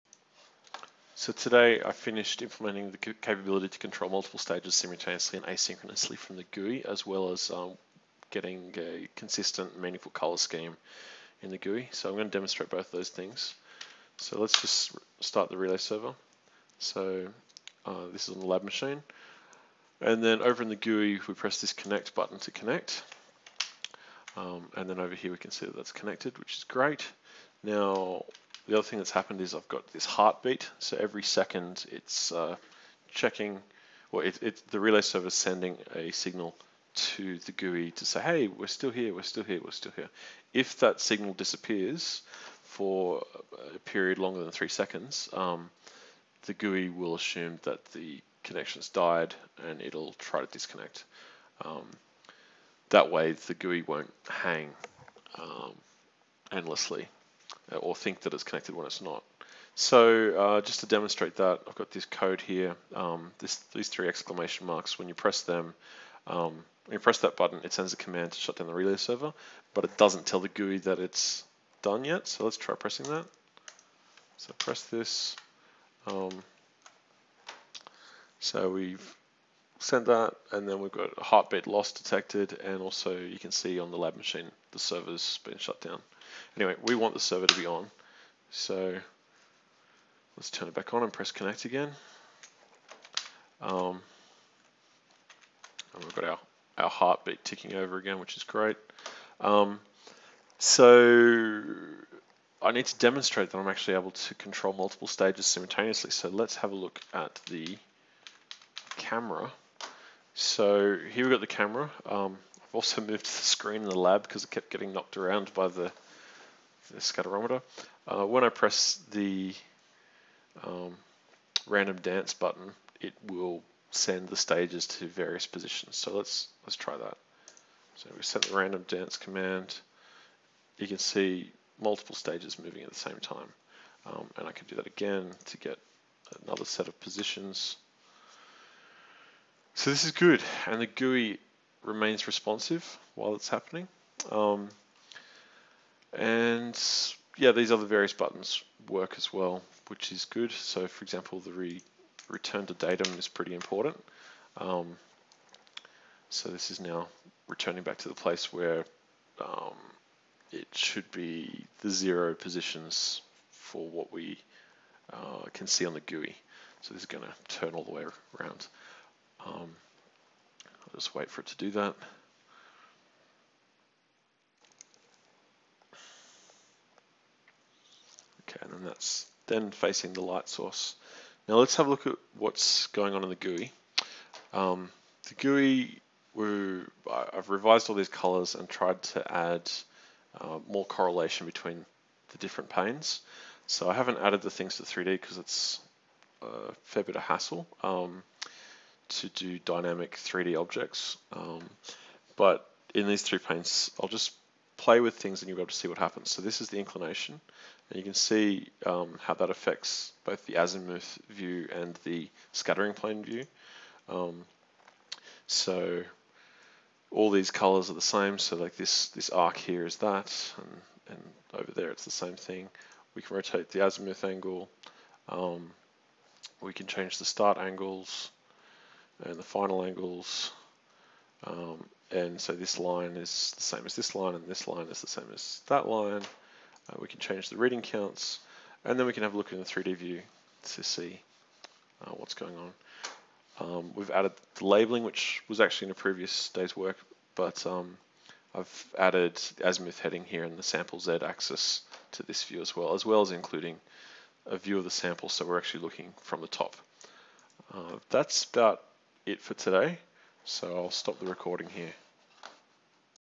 UNSW SEIT Scatterometer GUI Development Update 21
Update including:
"Implement capability to control multiple stages simultaneously and asynchronously from GUI"; and
"Implement consistent meaningful colours across 2D and 3D viewport content"